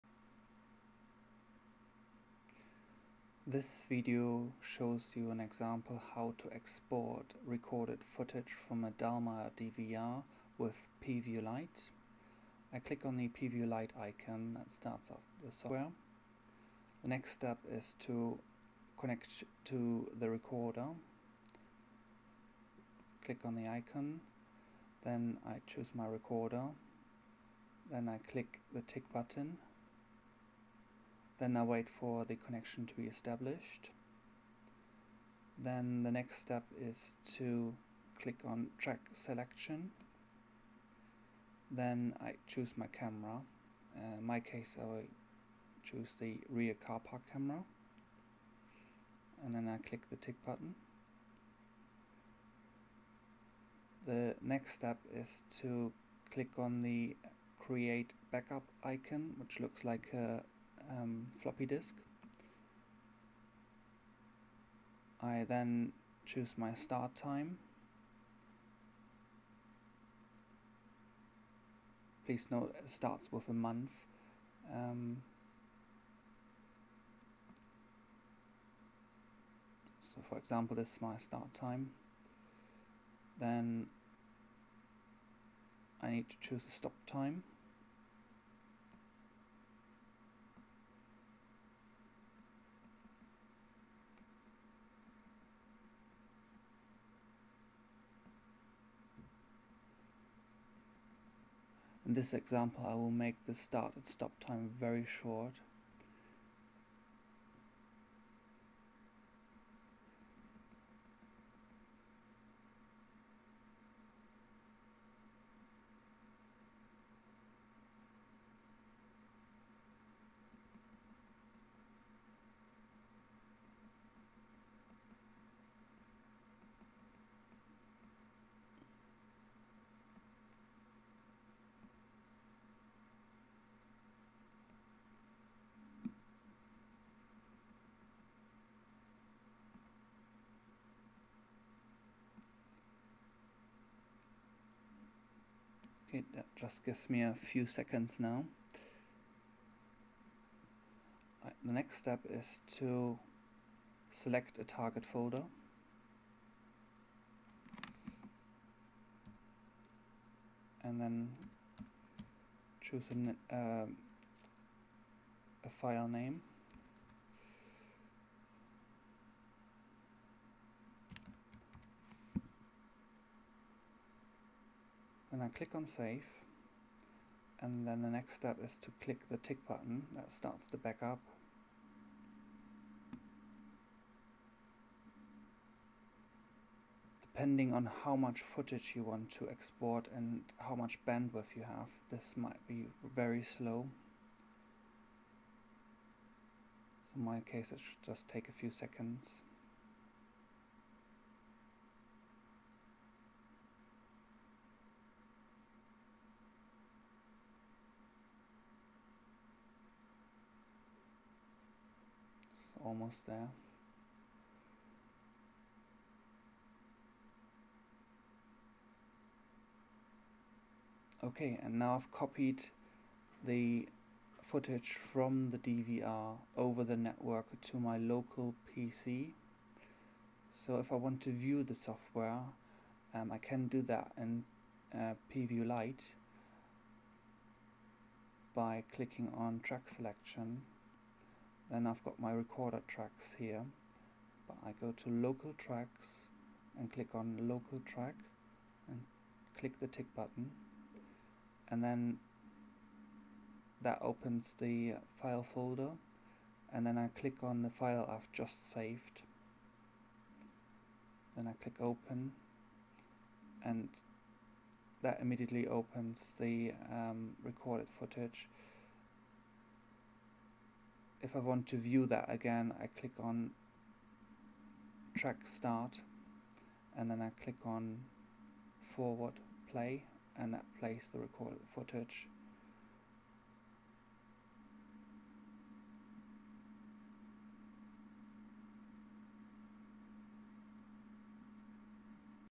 Dallmeier
How to export images from a Dallmeier DVR with PView Light